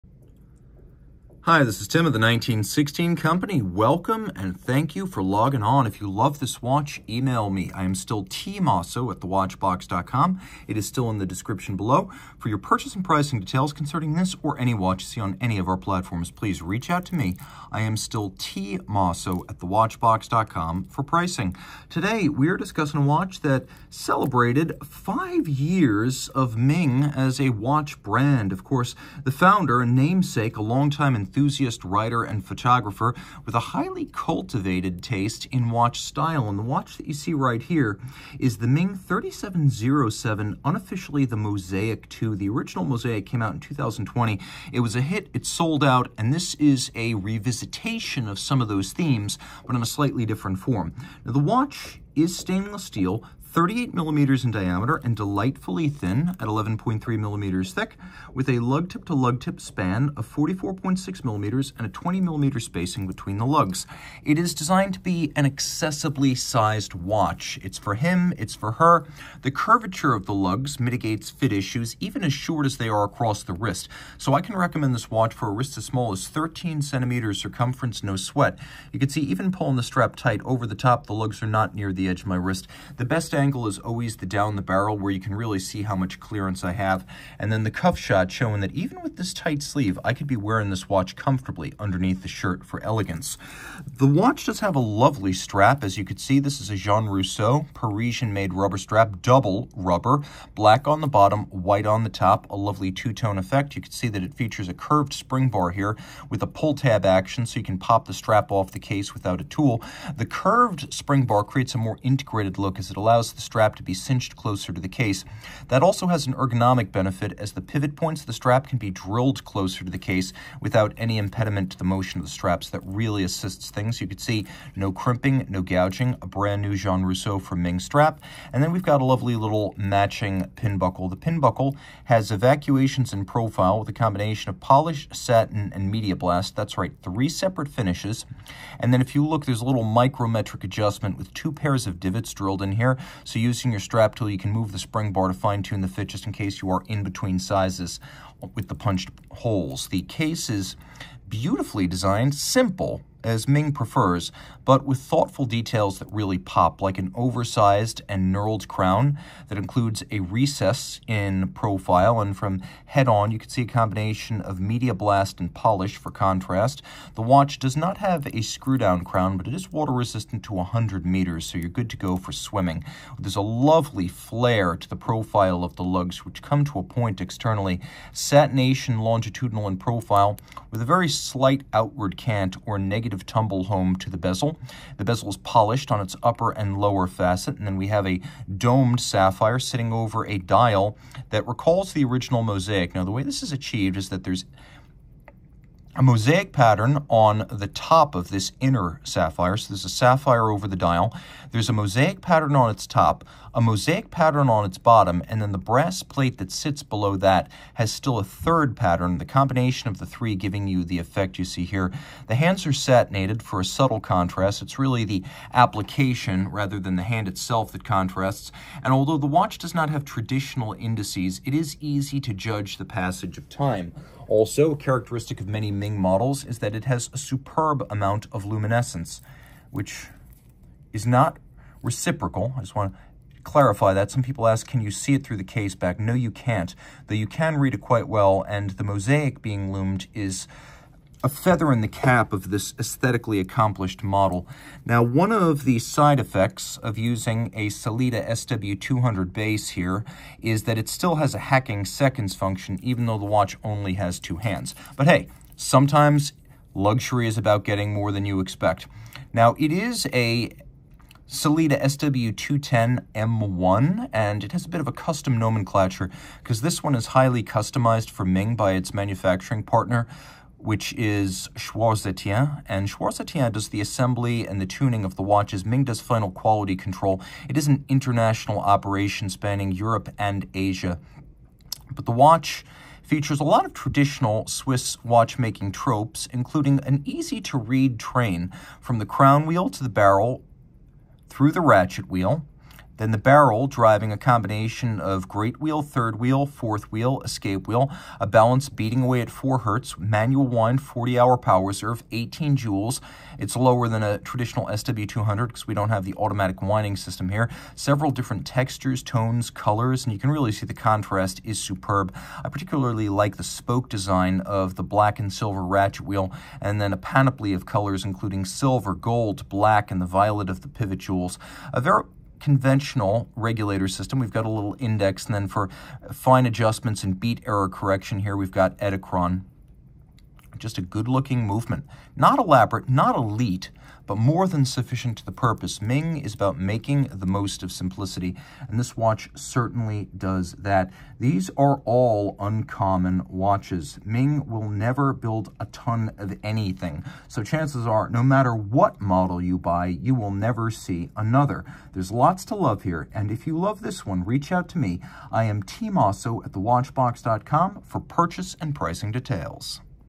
Ming 37.07 Ming Watch Review
SKU: 4908554

The Ming 37.07 is encased in 38mm of stainless steel surrounding a grey-black dial on a white leather strap. Features of this Ming 37.07 include hours and minutes.

For complete details, watch the full Ming 37.07 review.

WatchBox is now The 1916 Company.